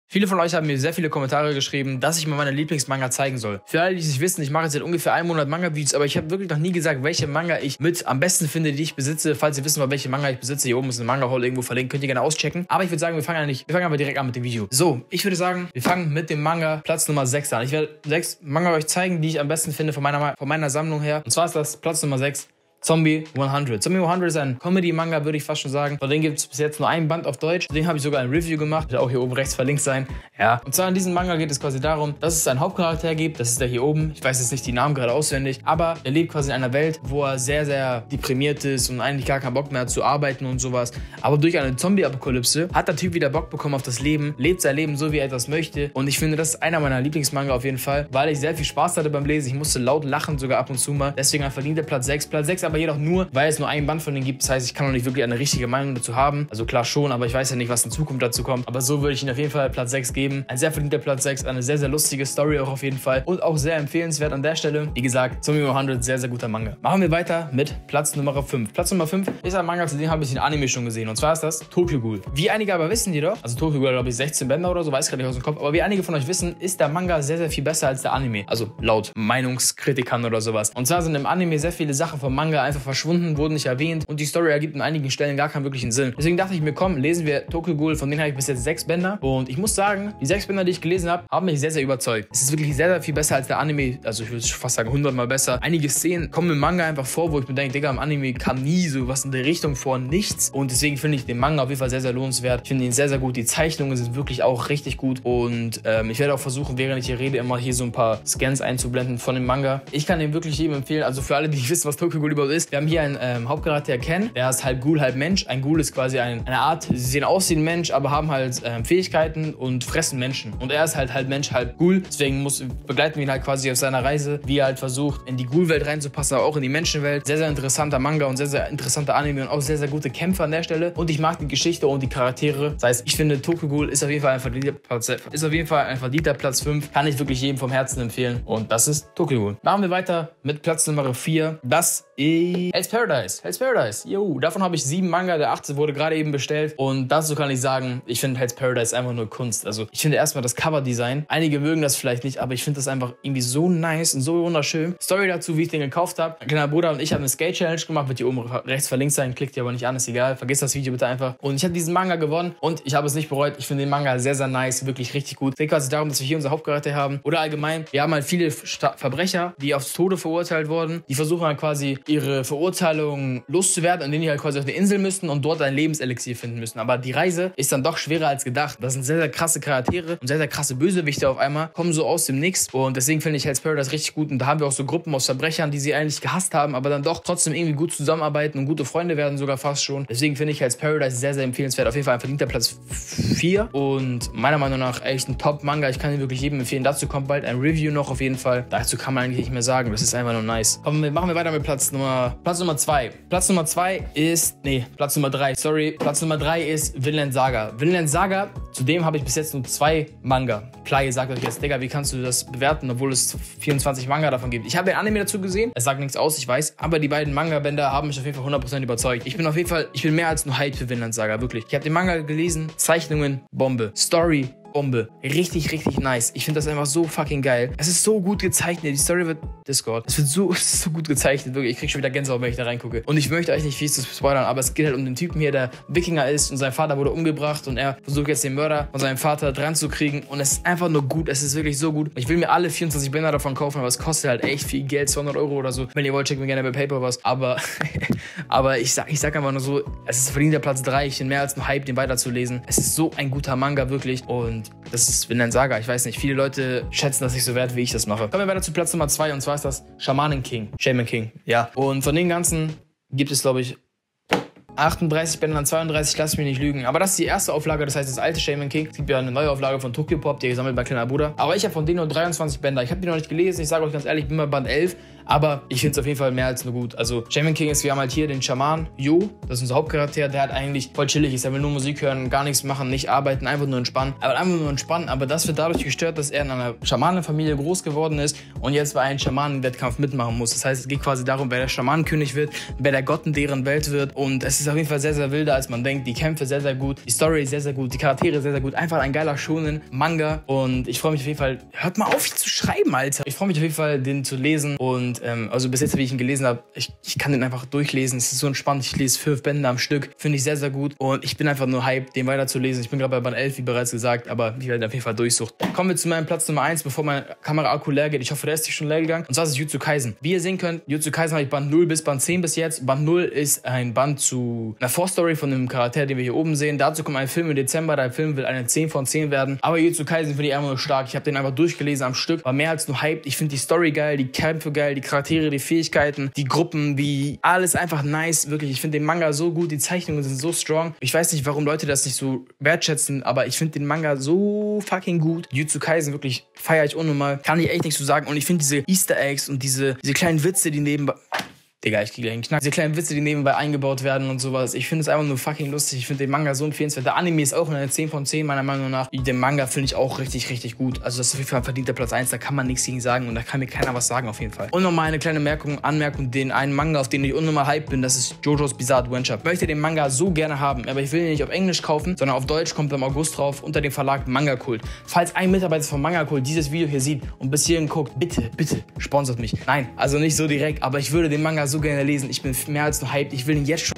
Die besten Manga ever! | Meine Top 6 Manga
In diesem Video, zeuge ich euch meine besten Manga. Einige von euch haben mit gefragt, was wären denn meine Top 5 Manga und genau diese zeige ich euch in diesem Video. Es waren Top 6 aber hey, wenn juckts.

Besten Manga / Favorit Manga / Manga Empfehlungen / Manga Collection